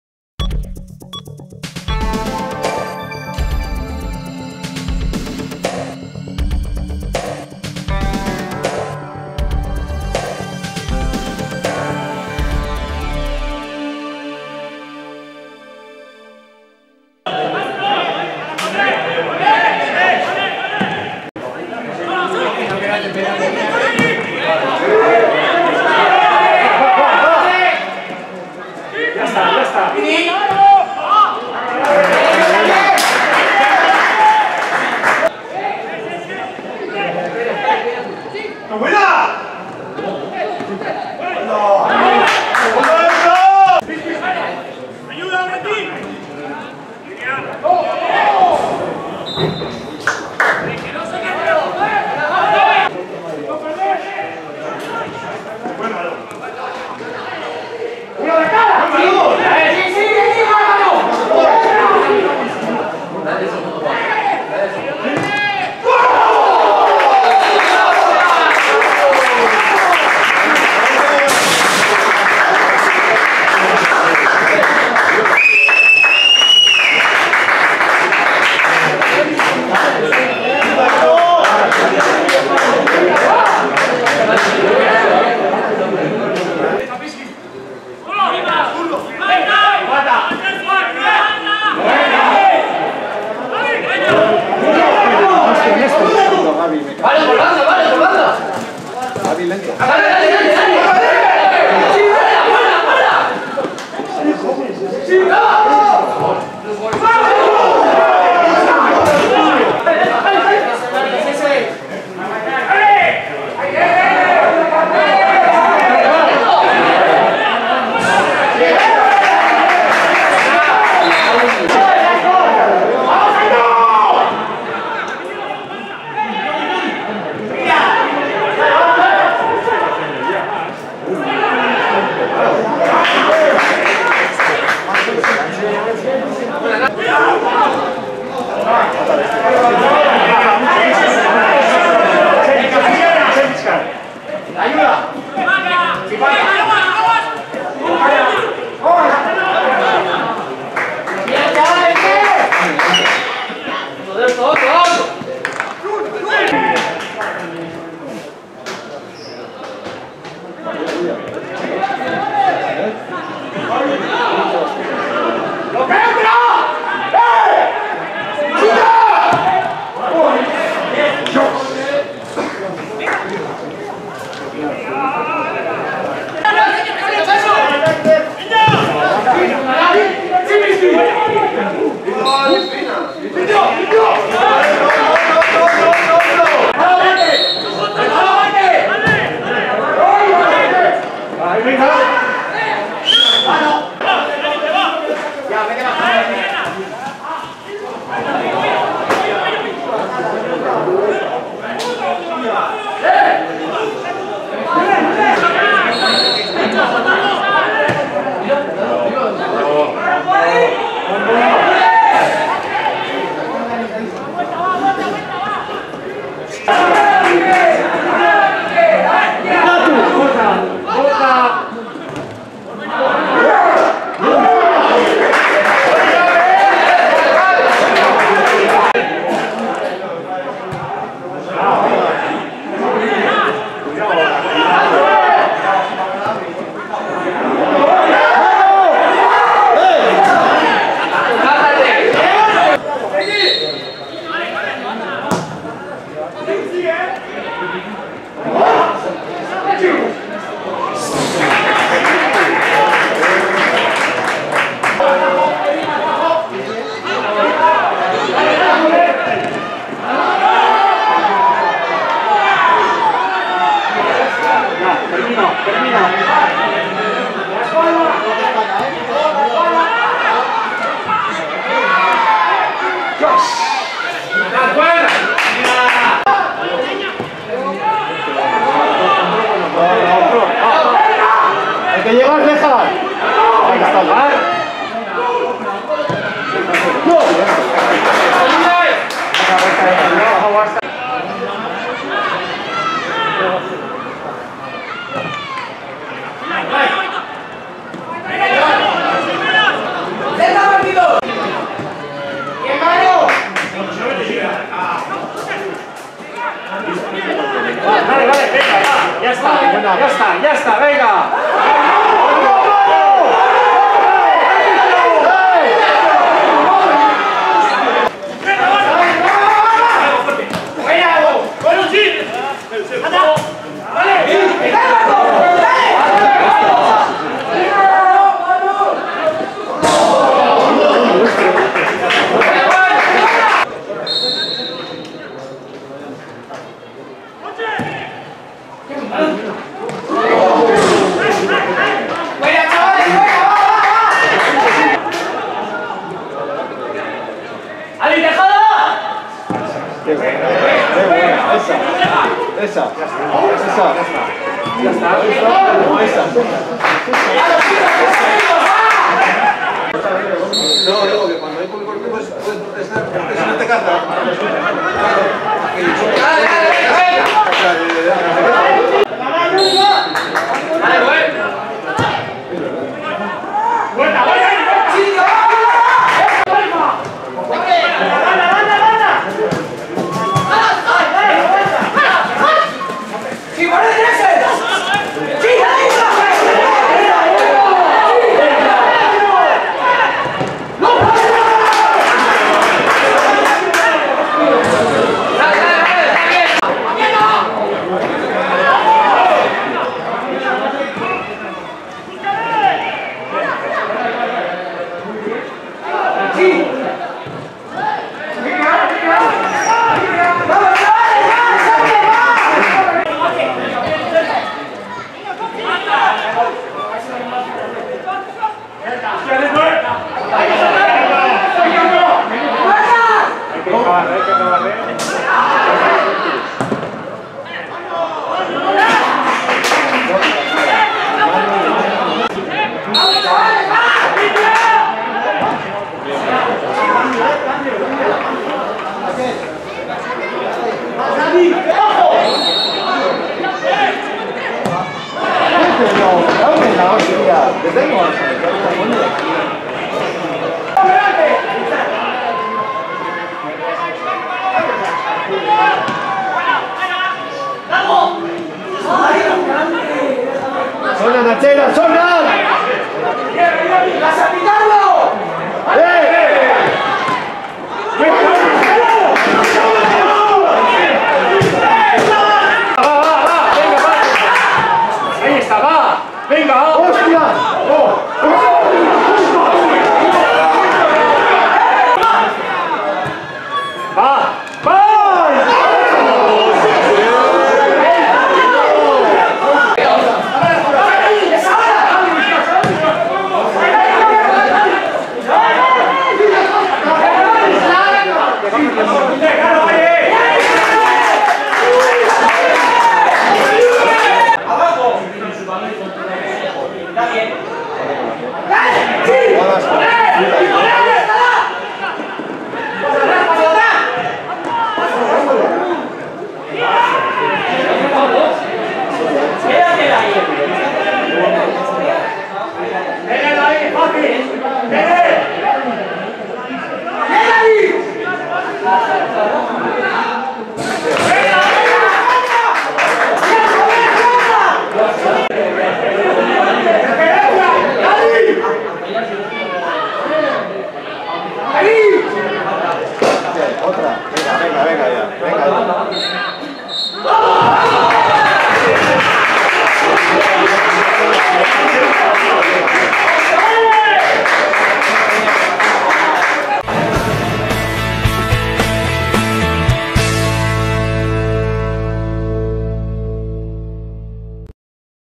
AURRERA K.E. 0:1 S.D. Deusto (Zaldupe, 2020/11/07)
2020ko Azaroaren 7an Ondarroako Zaldupe futbol zelaian jokatu zen partiduaren laburpena.